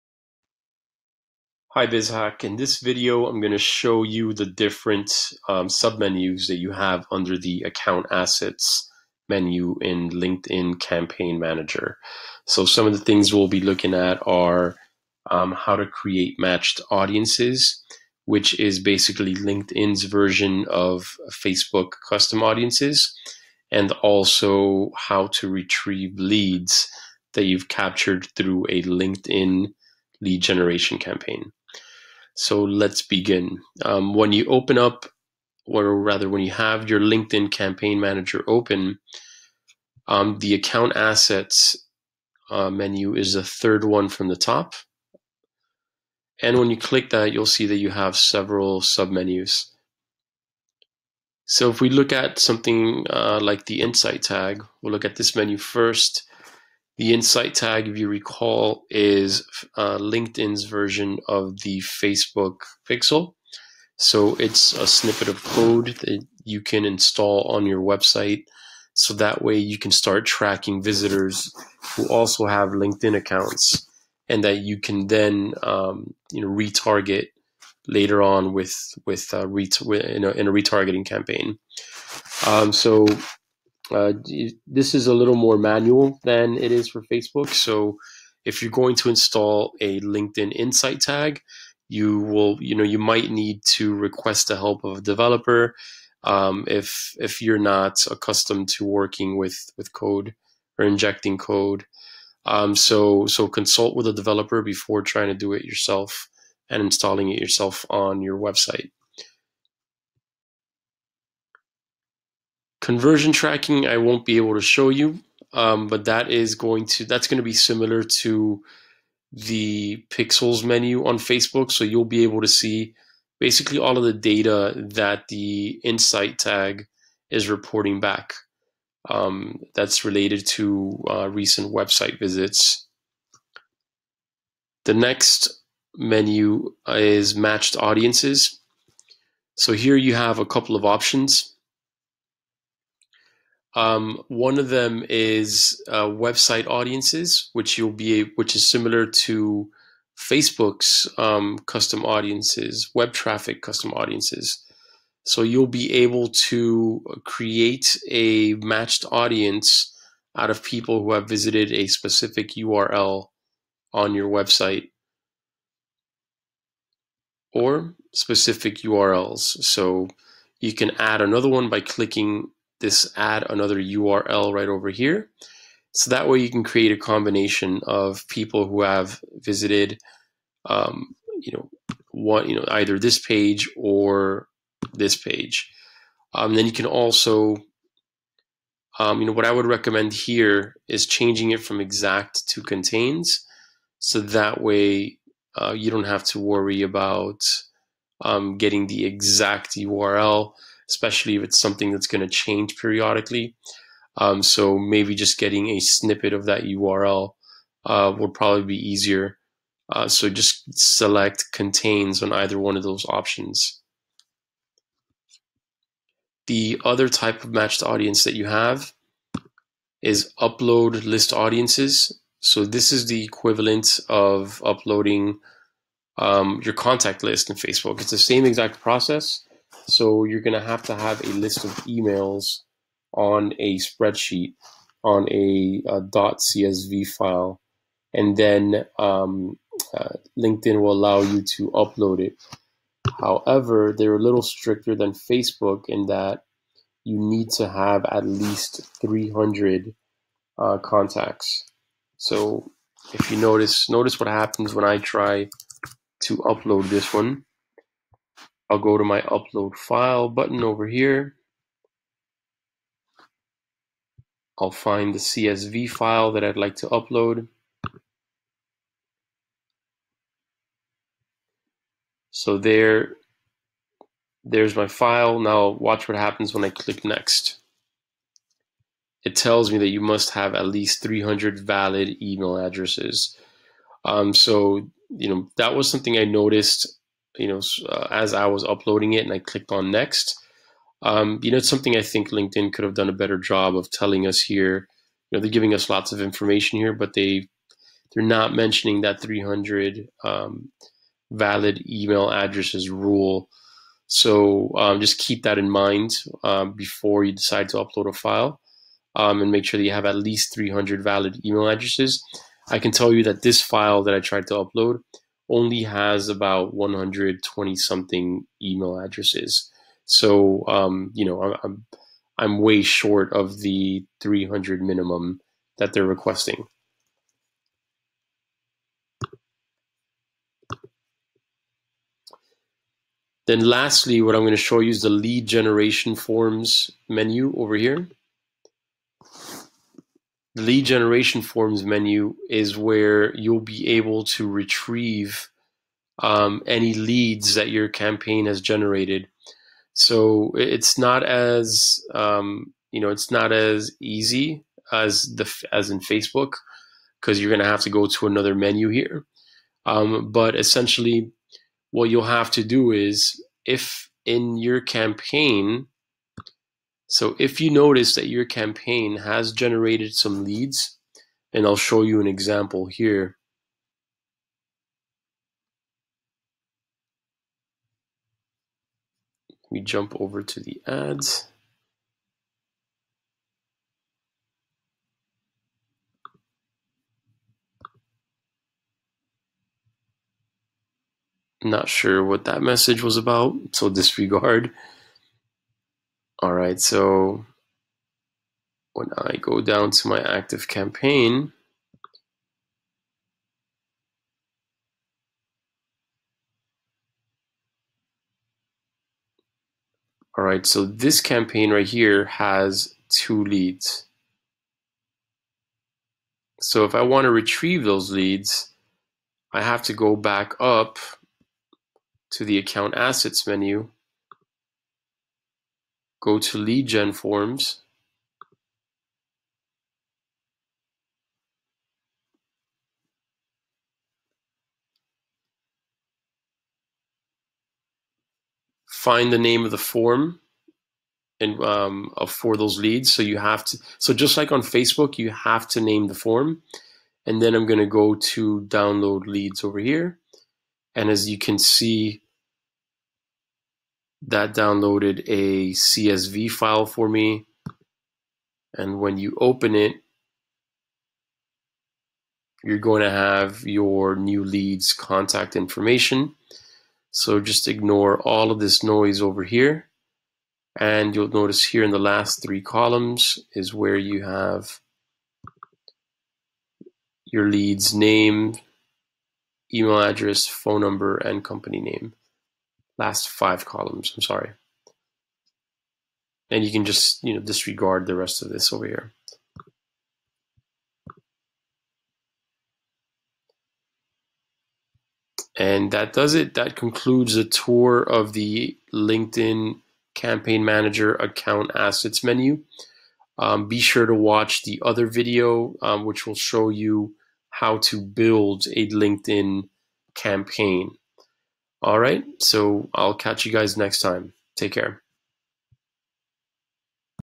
👨‍👩‍👧‍👦 HOW to CREATE matched audiences and retrieve leads through ☑️ LinkedIn Manager (2022)
A demonstration on how to create matched audiences and retrieve leads through LinkedIn Campaign Manager by Giovanni Insignares, lead instructor at BizHack Academy's Fundamentals of Digital Marketing Course in Miami. Giovanni will show you how to access different sub-menus under the account assets menu in the LinkedIn Campaign Manager. In this video, he will cover LinkedIn inside tags, matched audiences, uploading list audiences, and lead generation forms.